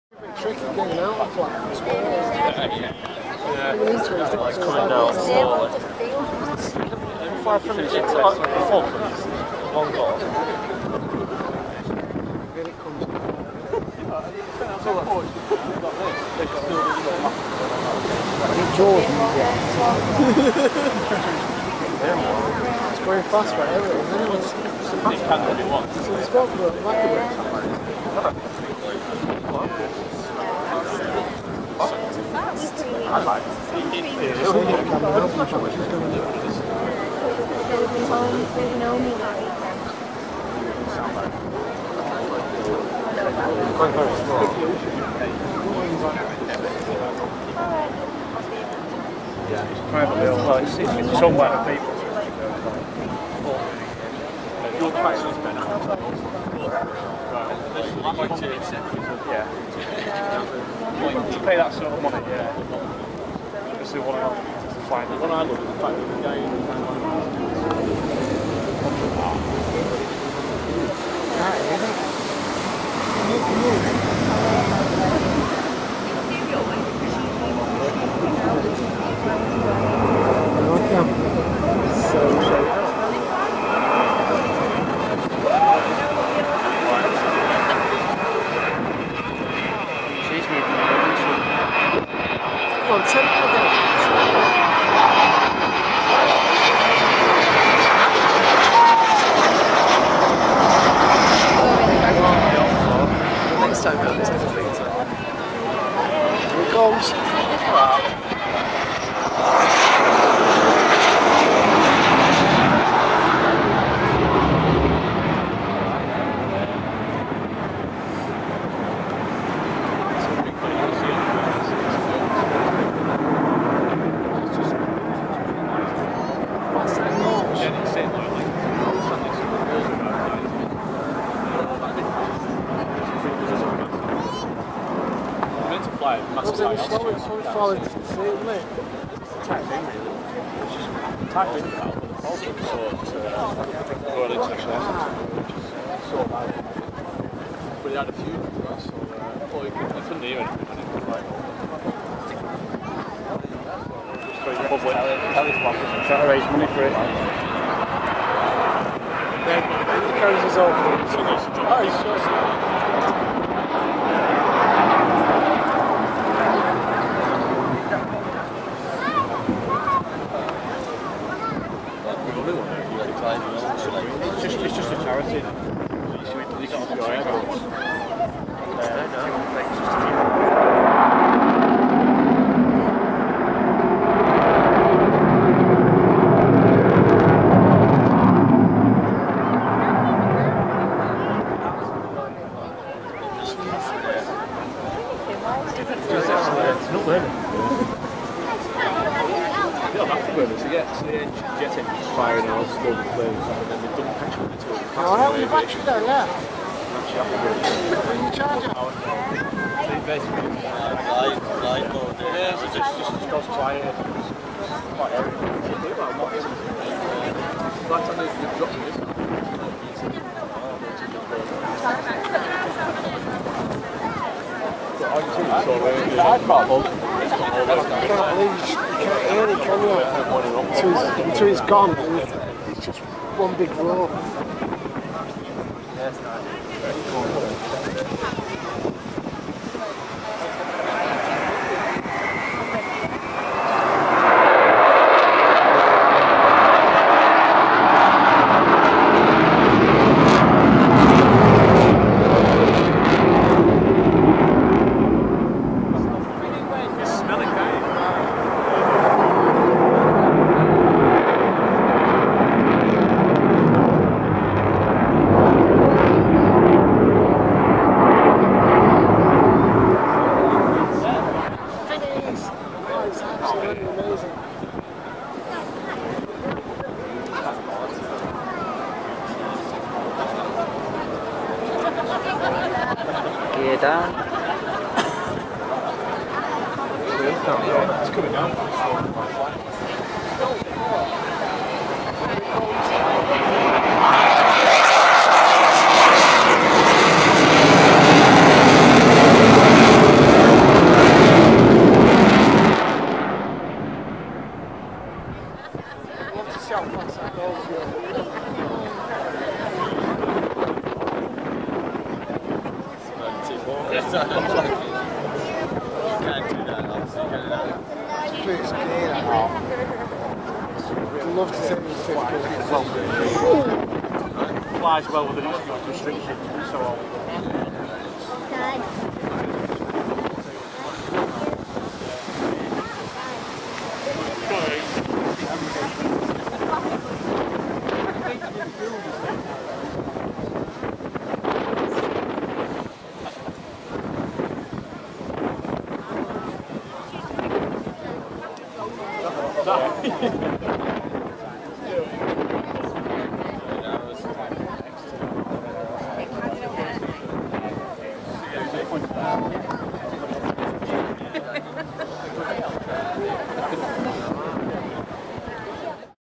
Blackpool AirShow 2013 Vulcan
wonderful entrance, low and slow in all her glory.